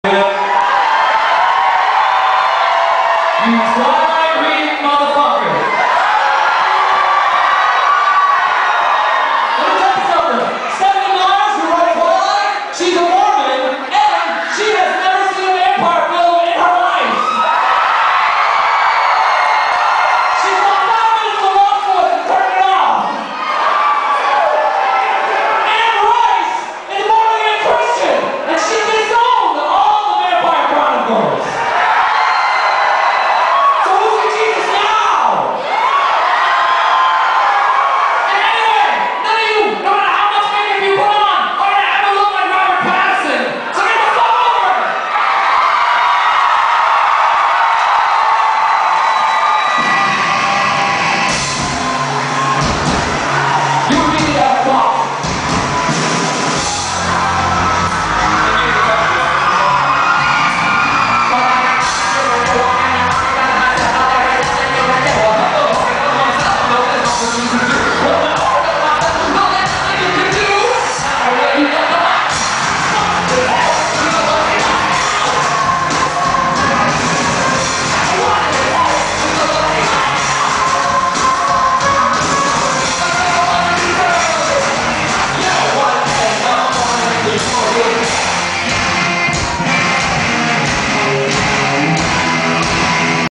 MSI Jimmy Talking.
Yea well he was talking about vampires and being hung over and then he starts to sing.